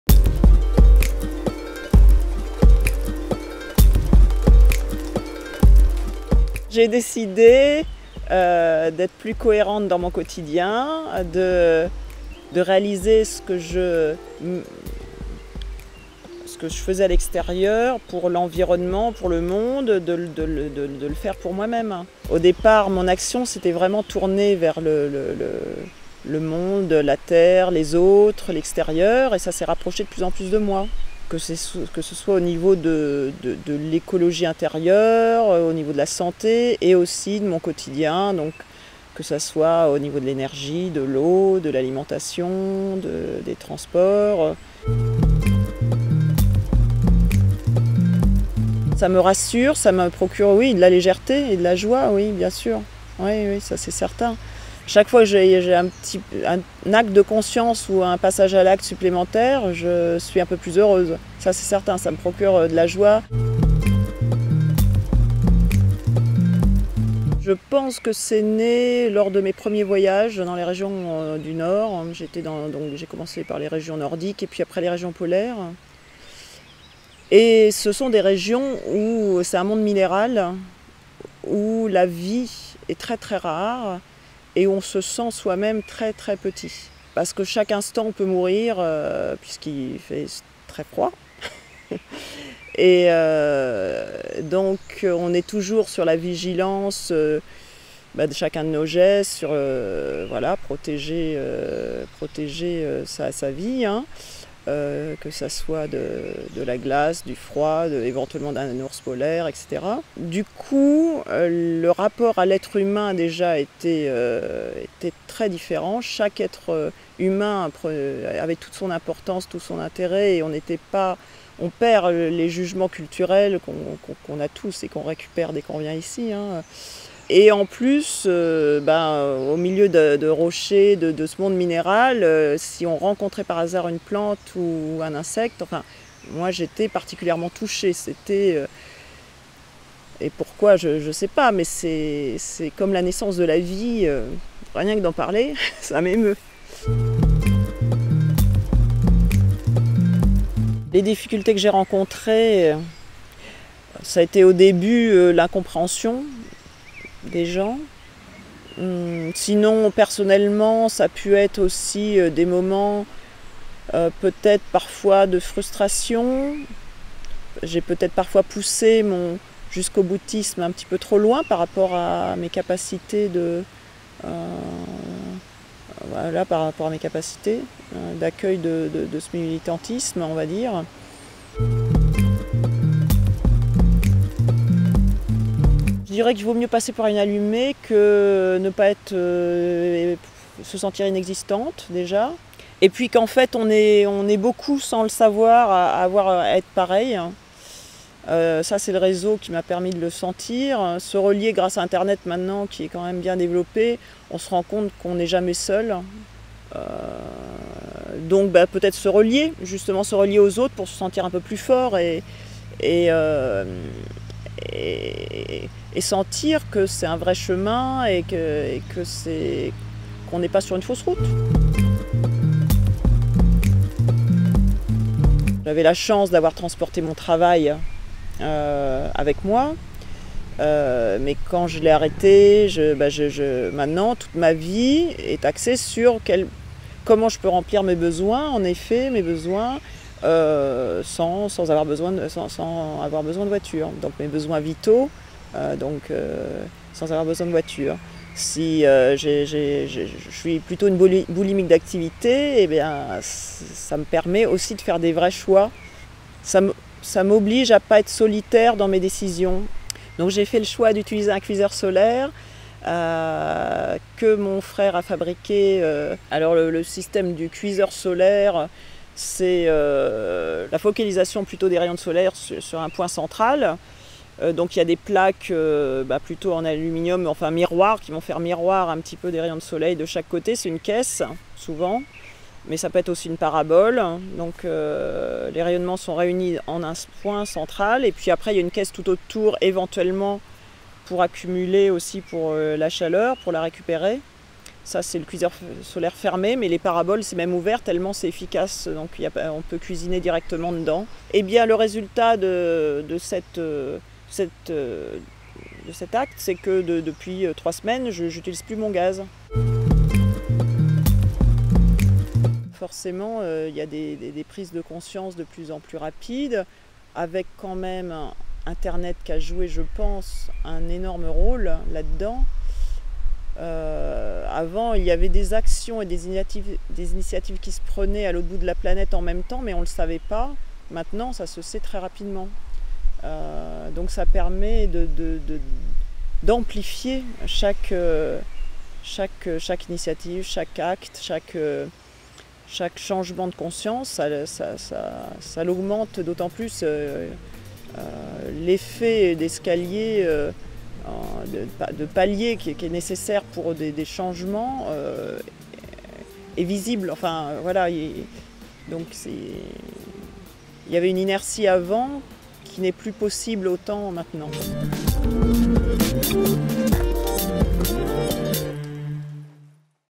Laurence vit dans la sobriété et en pleine conscience et ça la rend heureuse (VC)
Laurence a décidé d'aligner son mode de vie à son combat pour être plus cohérente avec elle-même. Elle a donc choisi la sobriété.
À retrouver sur onpassealacte.fr